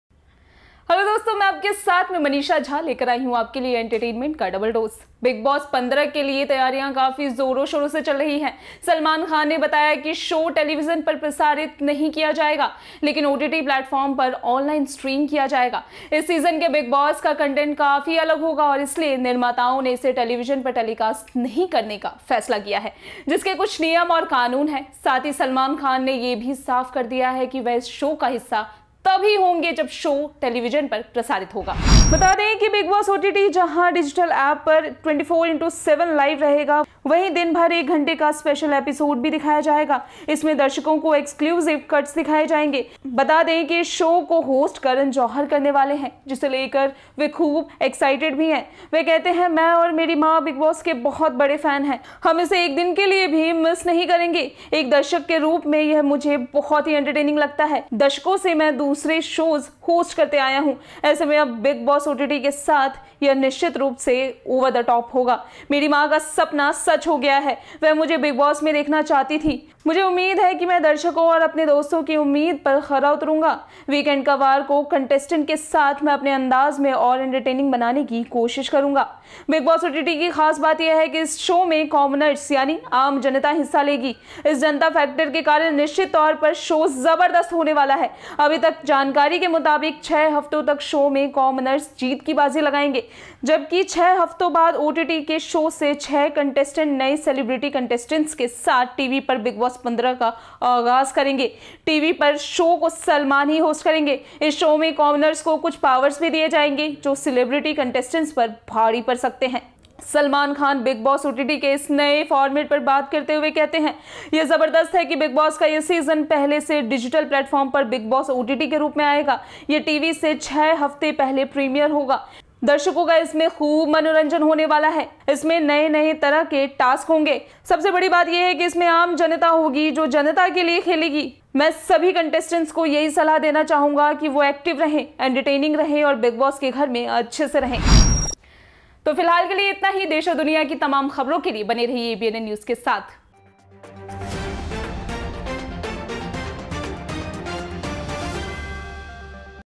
सलमान नहीं, ये film director होस्‍ट करेंगे ओटीटी पर 'बिग बॉस'।
बिग बॉस 15' के लिए तैयारियां काफी जोरों-शोरों से चल रही हैं. सलमान खान ने बताया कि शो टेलीविजन पर प्रसारित नहीं किया जाएगा, लेकिन ओटीटी प्लेटफॉर्म पर ऑनलाइन स्ट्रीम किया जाएगा. इस सीजन के बिग बॉस का कंटेंट काफी अलग होगा और इसलिए निर्माताओं ने इसे टेलीविजन पर टेलीकास्ट नहीं करने का फैसला किया है जिसके कुछ नियम और कानून हैं. साथ ही, सलमान खान ने यह भी साफ कर दिया है कि वह इस शो का हिस्सा तभी होंगे जब यह शो टेलीविजन पर प्रसारित होगा.